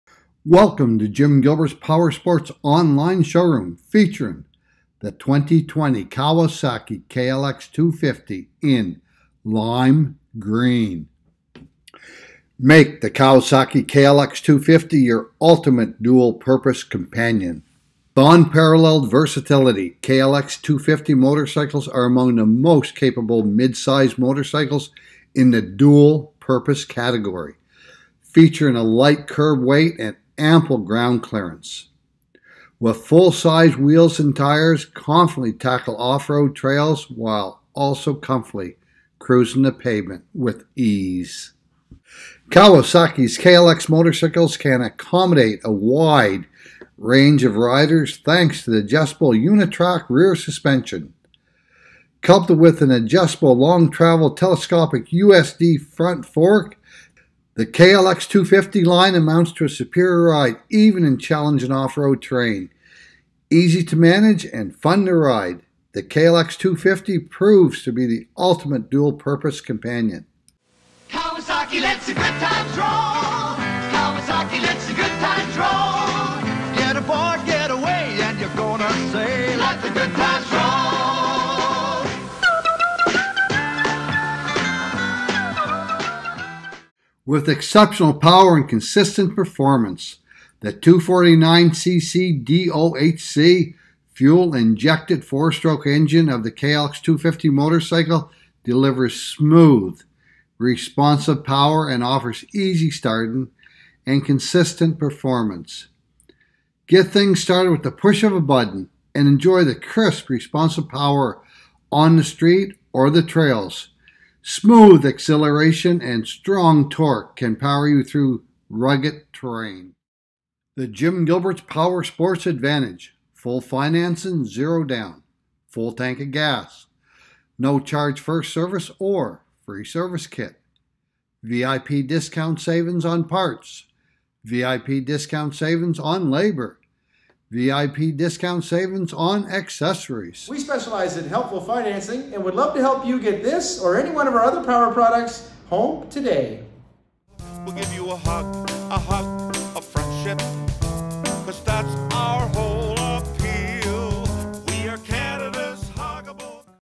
2020 KAWASAKI KLX 250, Lime Green | Jim Gilbert's PowerSports Fredericton
Welcome to Jim Gilberts PowerSports in Fredericton on-line showroom; The KLX250 motorcycle delivers serious on- and off-road riding performance that appeals to riders of various skill levels. This dual-purpose motorcycle boasts a 249cc engine and provides a superior ride for confidently navigating through city streets or off-road trails.
49CC, ONE-CYLINDER, DOHC, FOUR-STROKE ENGINE
EXCEPTIONAL GROUND CLEARANCE
ADJUSTABLE FRONT AND REAR SUSPENSION
FUEL INJECTION SYSTEM
Low seat height
Comfortable ergonomics
Sharp, aggressive KX-inspired bodywork

Jim Gilbert's PowerSports,
402 St Marys St #2, Fredericton, NB E3A 8H5